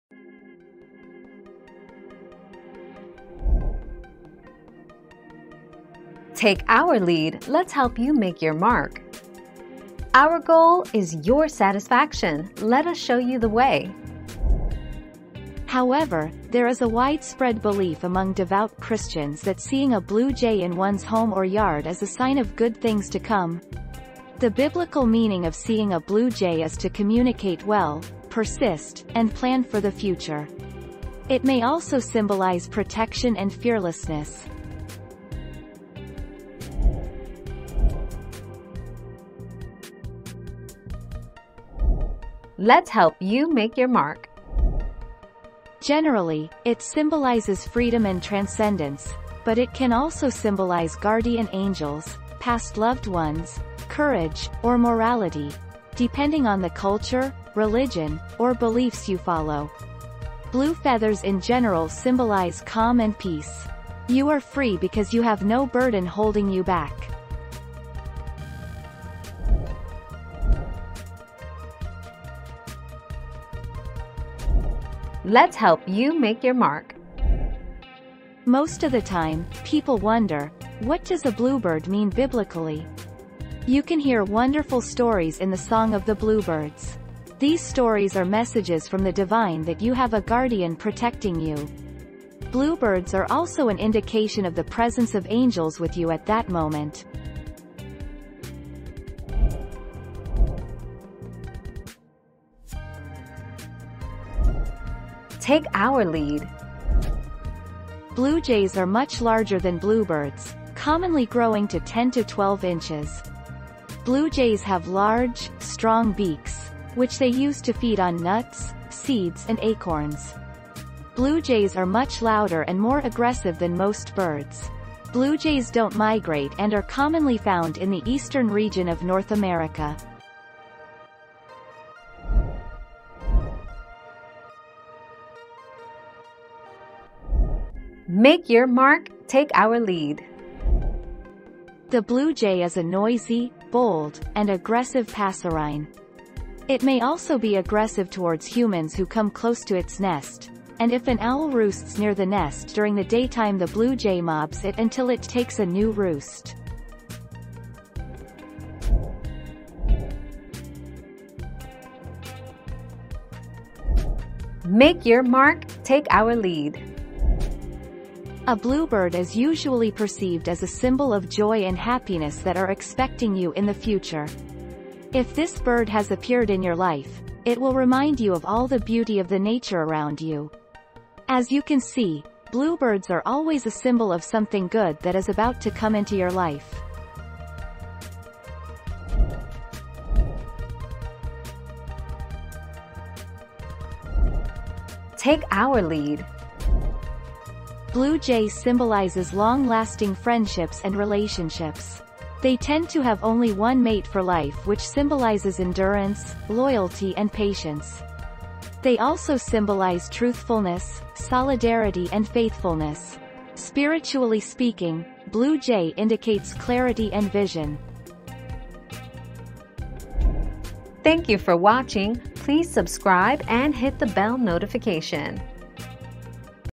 What does it mean when you see a blue jay in your yard?
00:00 - What does it mean when you see a blue jay in your yard?
00:38 - What do blue feathers mean?
01:11 - What does a bluebird mean biblically?
01:40 - What is the difference between a bluebird and a blue jay?
02:13 - Are Blue Jays mean to humans?
02:40 - What does seeing a blue bird mean?
03:10 - WHAT DO BLUE JAYS mean spiritually?

Laura S. Harris (2021, May 7.) What does it mean when you see a blue jay in your yard?
    AskAbout.video/articles/What-does-it-mean-when-you-see-a-blue-jay-in-your-yard-242023